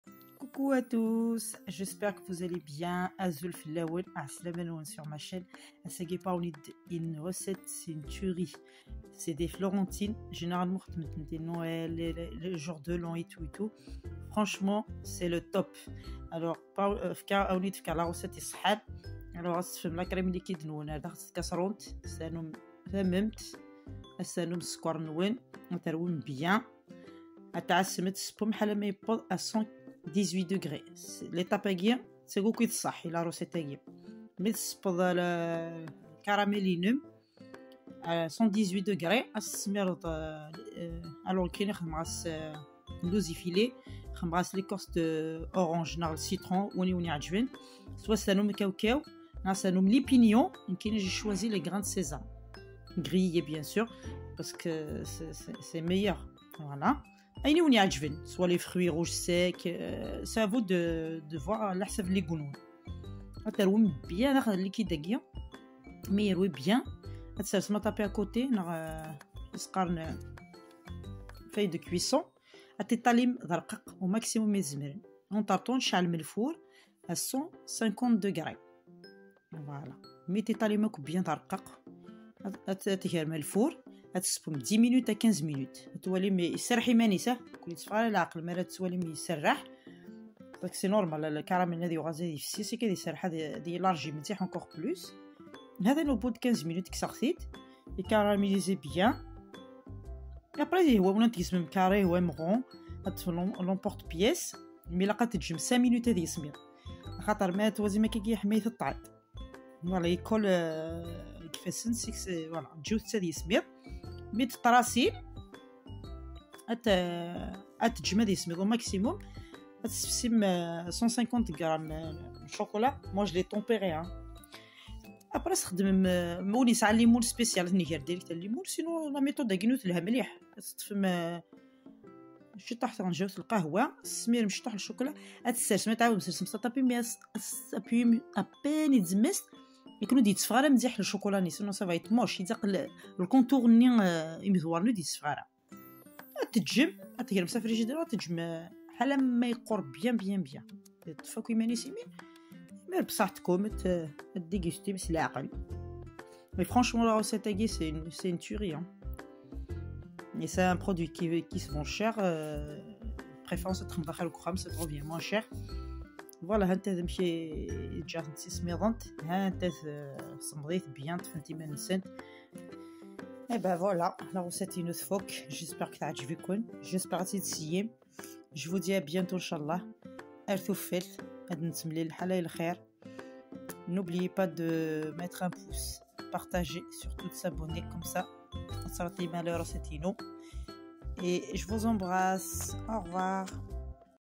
la vraie recette des florentins 😋😋😋❤
recette de florentine:
on mélange 150 g de sucre avec 50 g de miel et 160 g de crème liquide cuire jusqu'à atteindre 118 degrés puis mélanger 100 g de écorce d'orange au citron + 100 g d'amandes effilées 50 g de Pinon au cacahuète moi j'ai mis des grains de sésame grillé bien mélanger!!
et aller sur un tapis au papier de cuisson une fine couche préchauffer le four à 150 cuir  minutes à 15 minutes
laisser refroidir 5 minutes
fait des ronds avec un emporte-pièce puis fait fondre 150 g de chocolat noir
mettre un petit peu de chocolat dessus et appuyez avec la peau du doigt au milieu